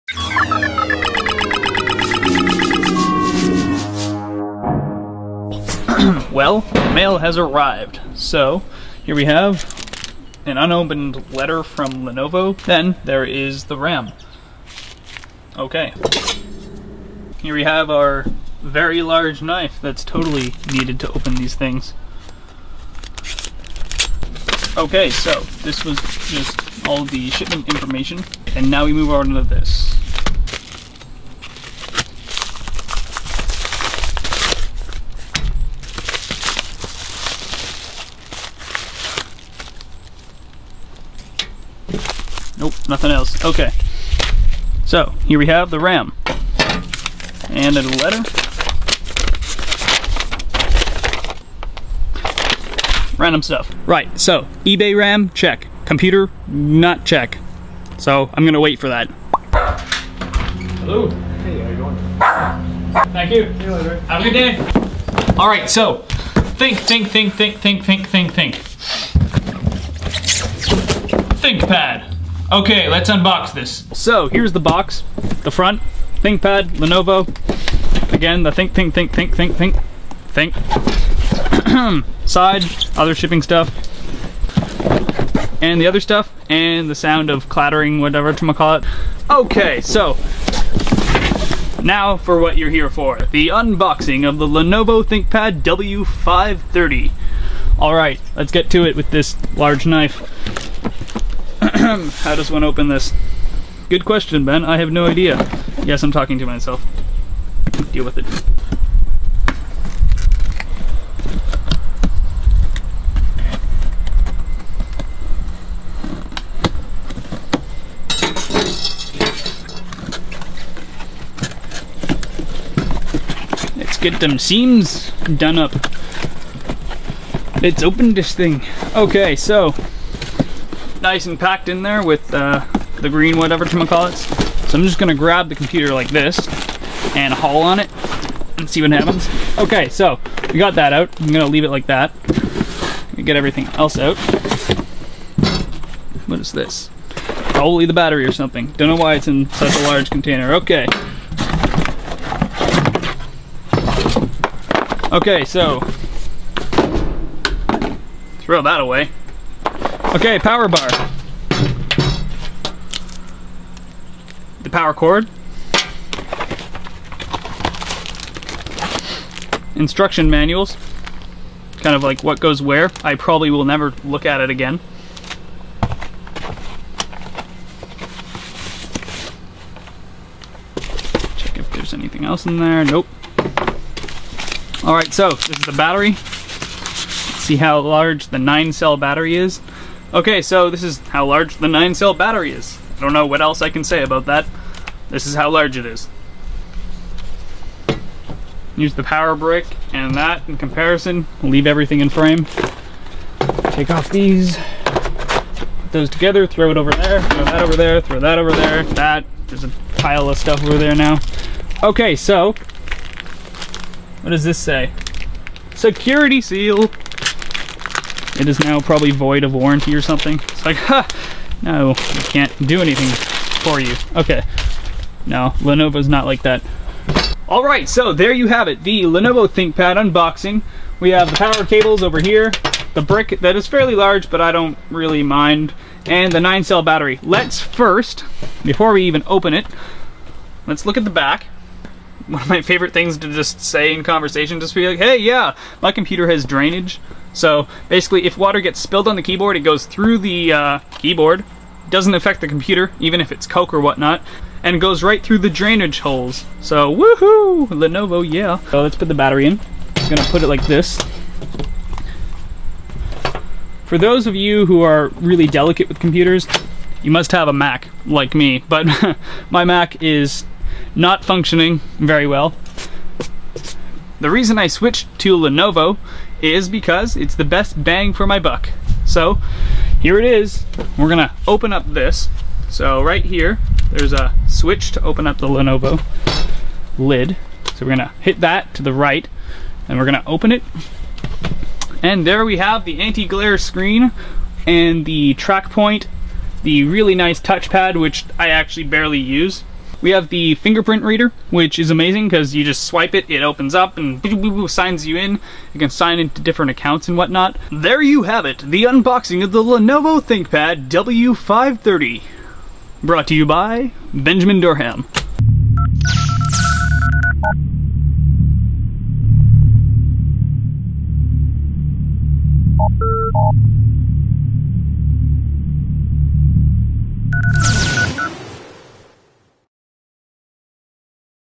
Complete Lenovo ThinkPad W530 Unboxing
For those of you that are interested, it took 16 days for everything to ship both from Lenovo and eBay.